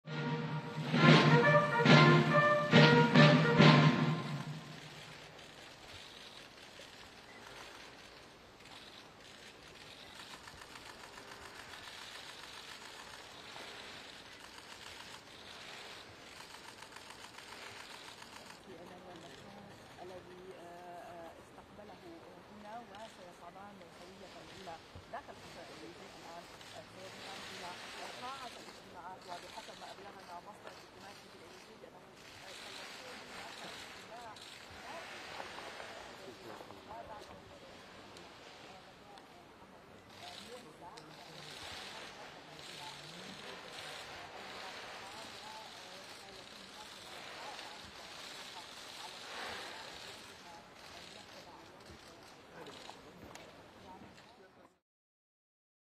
Emmanuel Macron receives Egyptian President Abdel Fattah Al-Sissi | AFP
French President Emmanuel Macron welcomes his Egyptian counterpart Abdel Fattah Al-Sissi to the Elysée Palace for a working lunch.